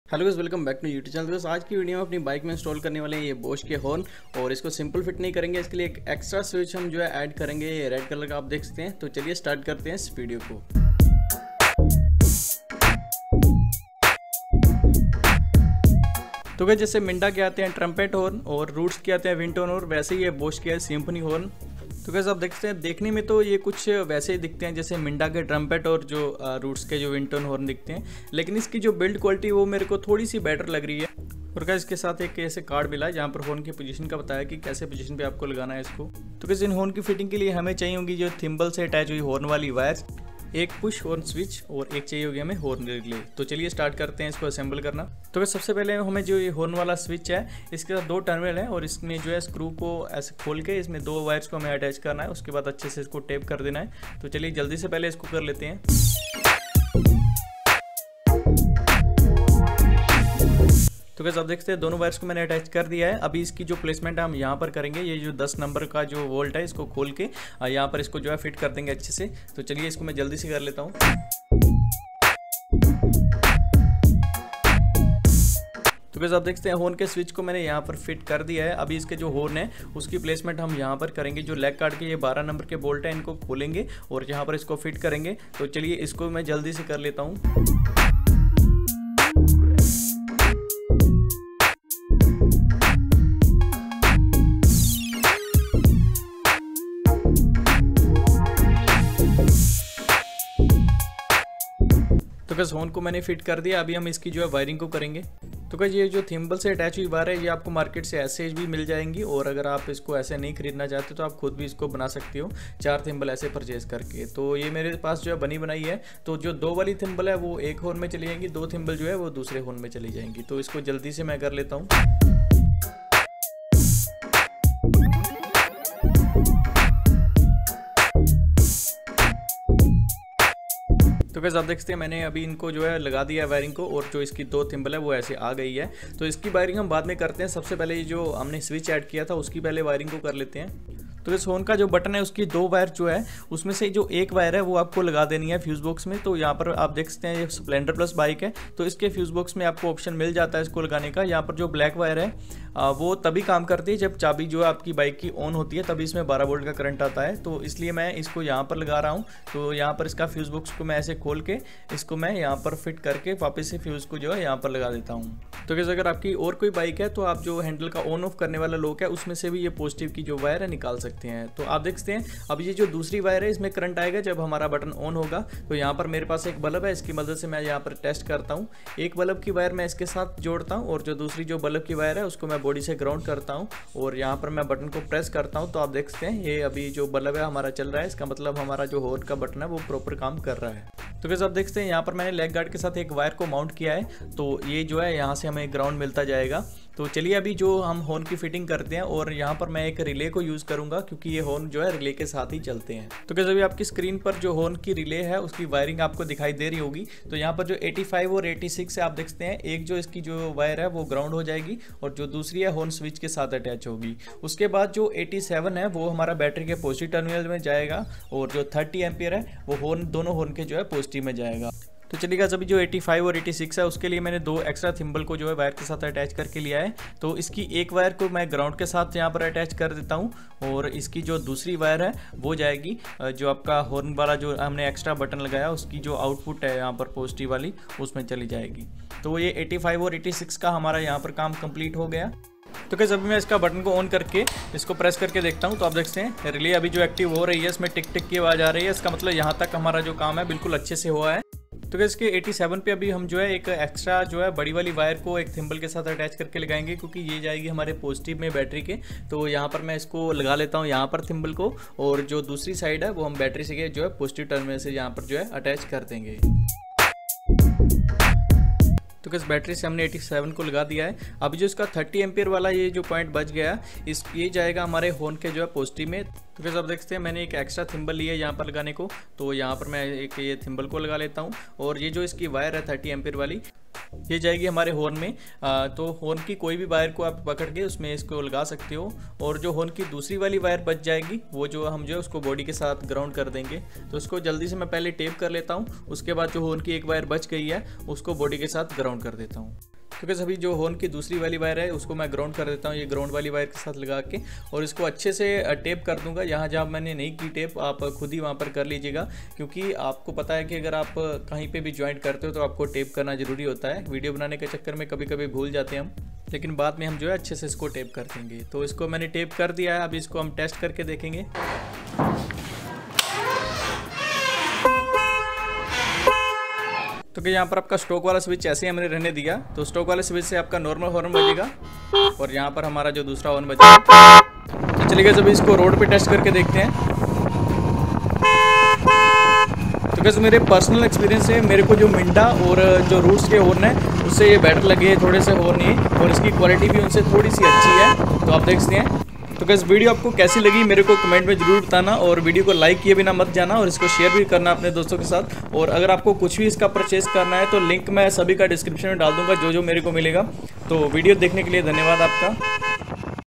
Dual tone Bosch horn switch
in this video i will install "Bosch F002H10028 Symphony Horn" with extra horn switch and relay on my hero splendor plus.

buy link
bosch horn

horn switch

horn relay